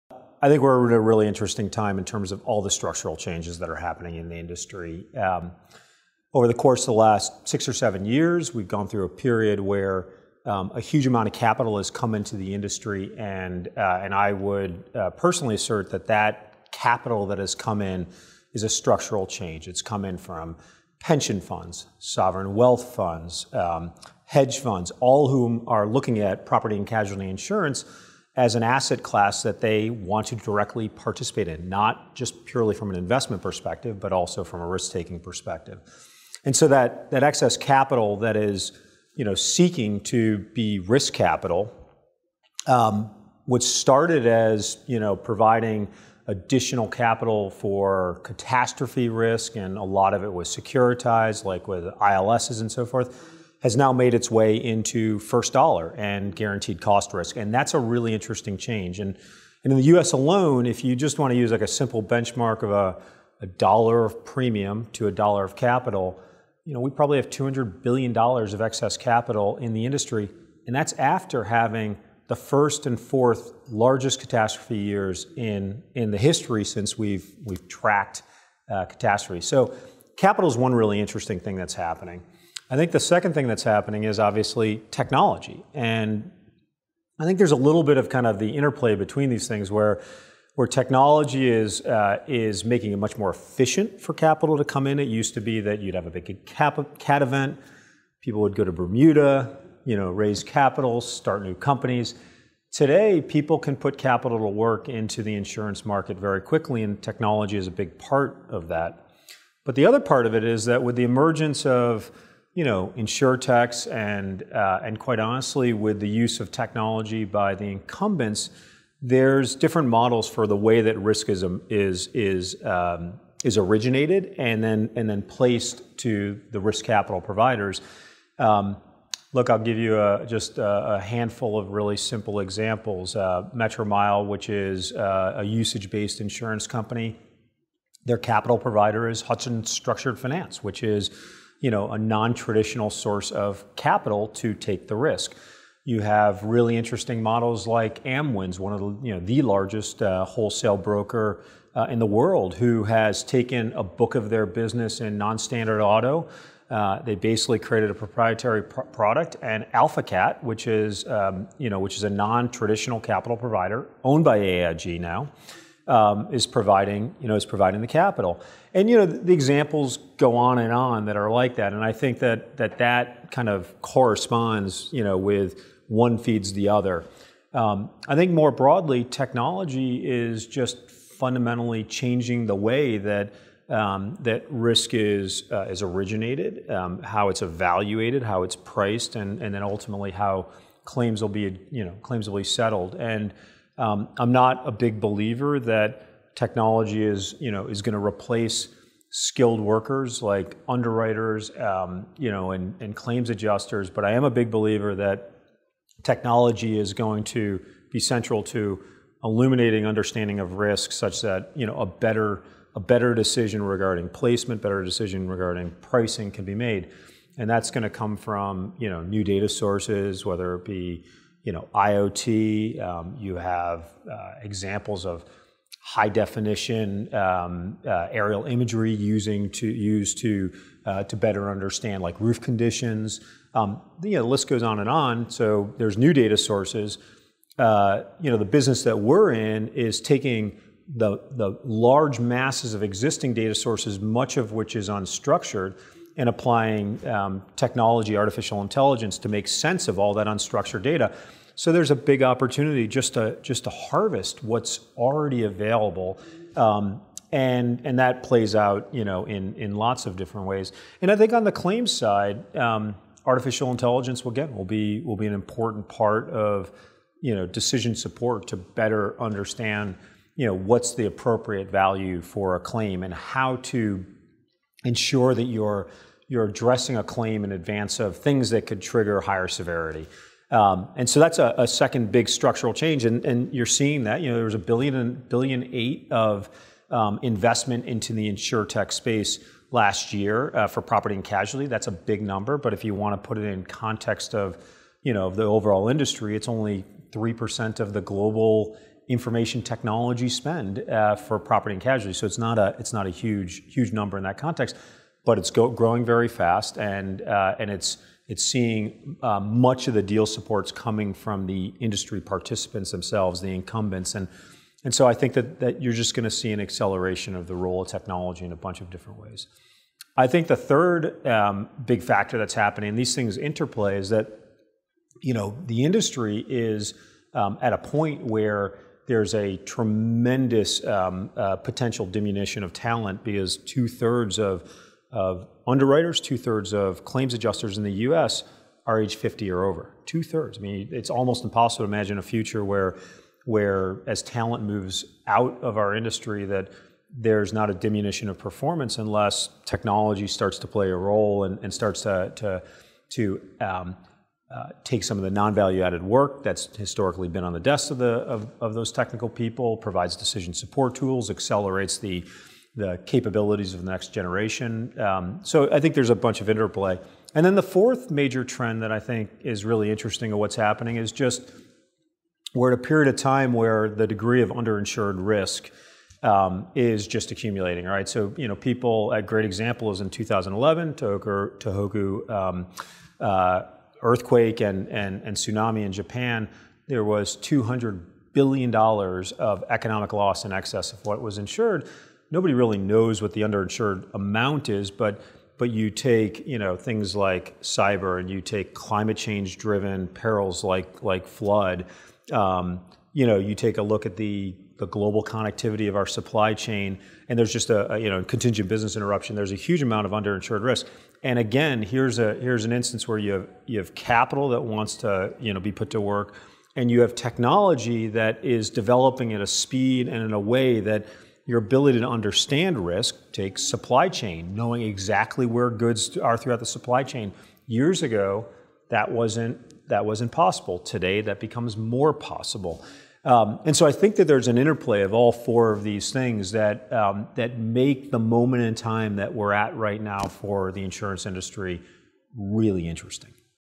Structural Changes in the Insurance Industry – Andrew Robinson
Groundspeed Co-CEO Andrew Robinson talks structural changes in the insurance industry at the 2019 Future of Risk conference held by The Institutes Risk & Insurance Knowledge Group.